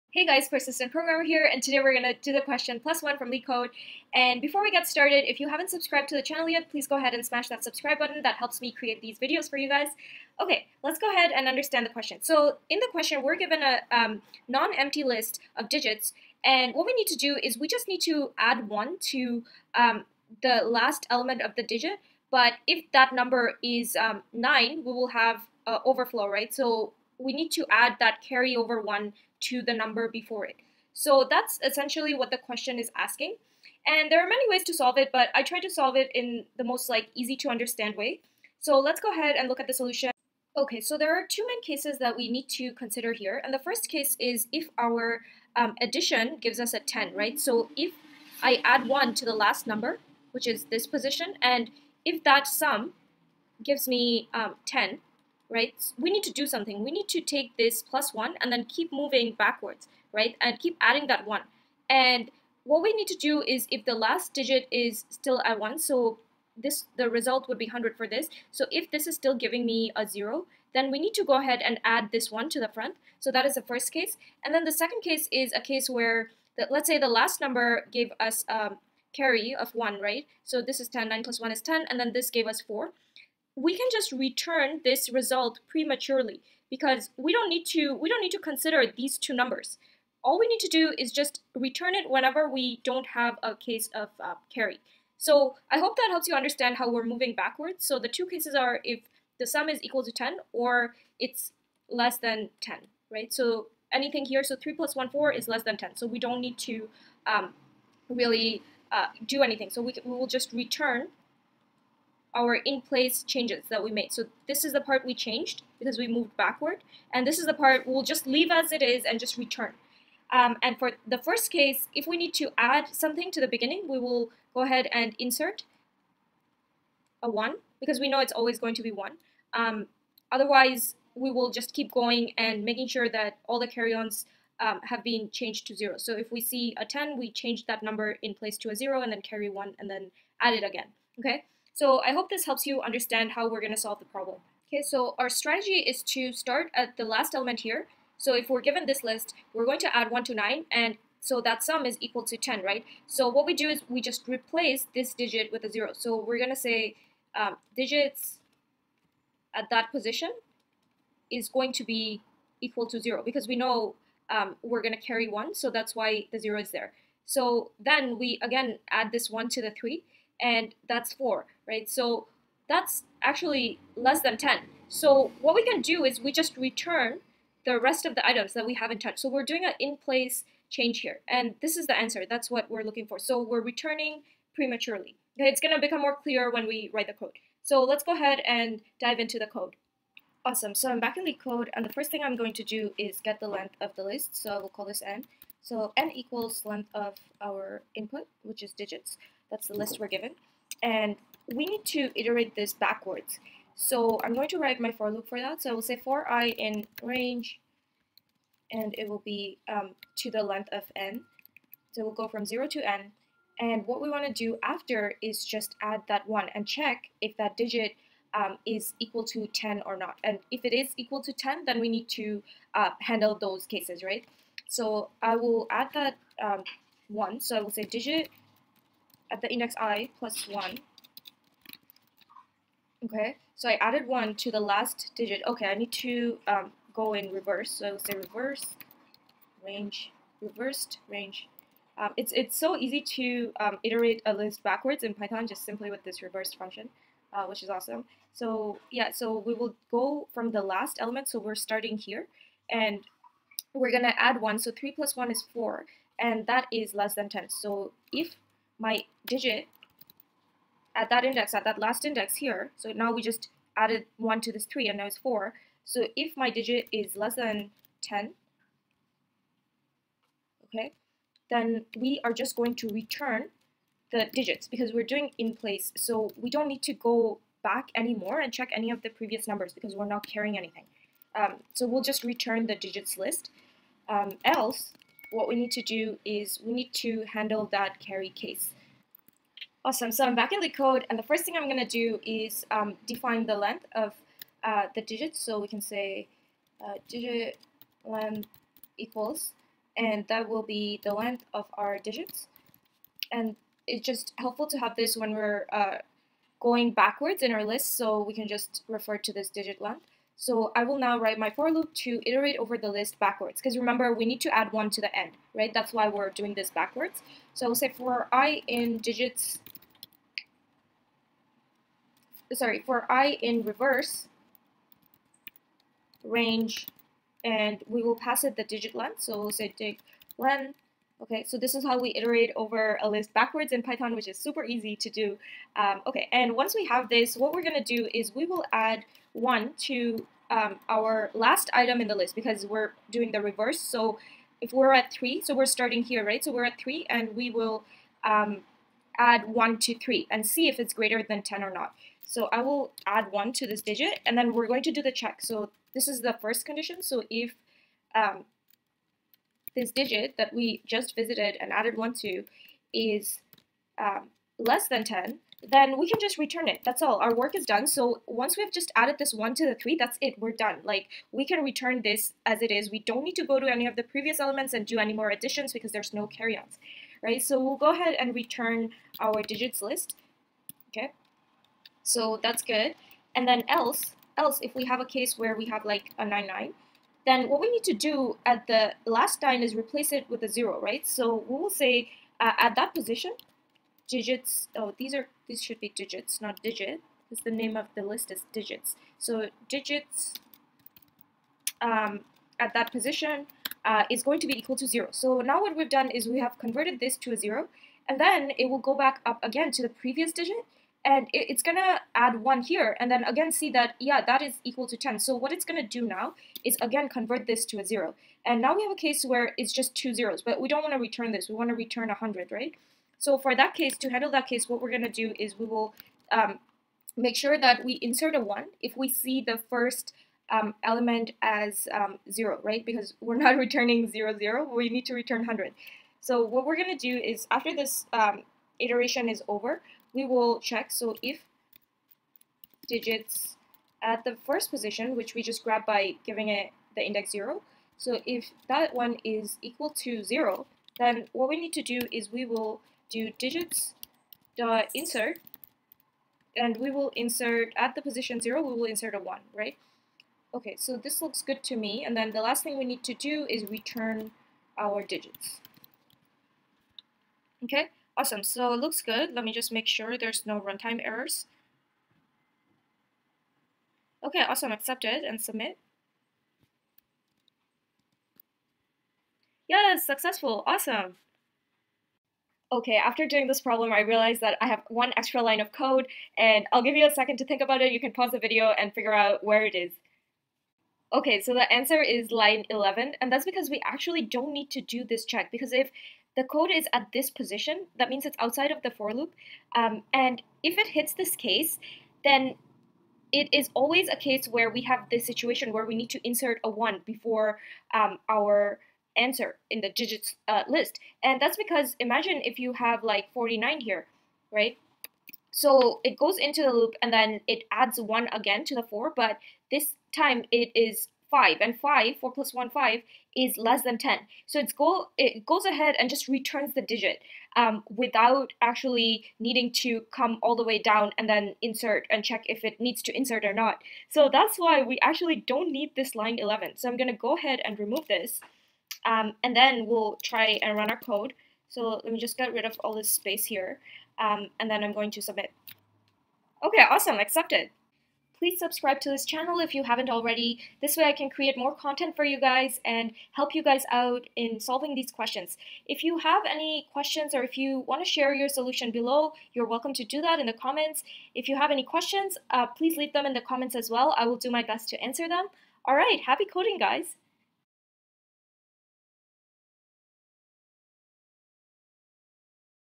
Plus One Python Leetcode | For Loop Python | Algorithm explained
🔴 Question with Example -
66. Plus One
Given a non-empty array of digits representing a non-negative integer, plus one to the integer.

The digits are stored such that the most significant digit is at the head of the list, and each element in the array contain a single digit.

You may assume the integer does not contain any leading zero, except the number 0 itself.

Example 1:

Input: [1,2,3]
Output: [1,2,4]
Explanation: The array represents the integer 123.